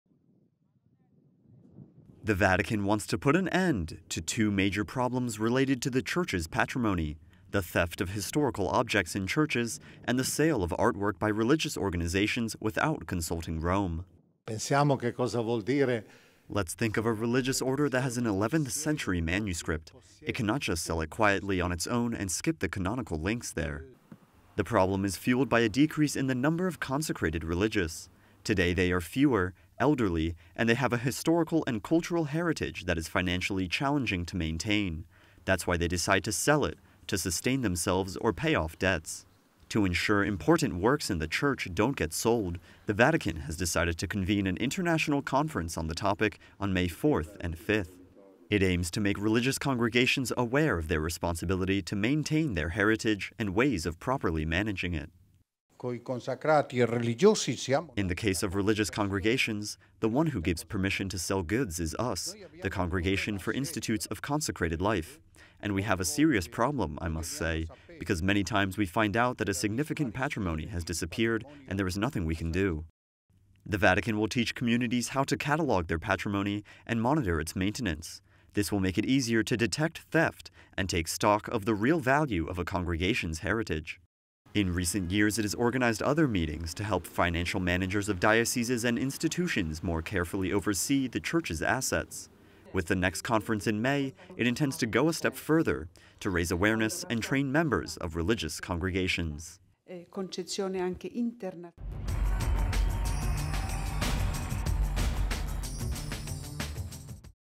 Vatican to train religious communities to not give up their rich historical patrimony
It will convene a congress on May 4 and 5 and will insist on the importance of cataloging important historical items.

The Vatican wants to put an end to two major problems related to the Church's patrimony: the theft of historical objects in churches and the sale of artwork by religious organizations without consulting Rome.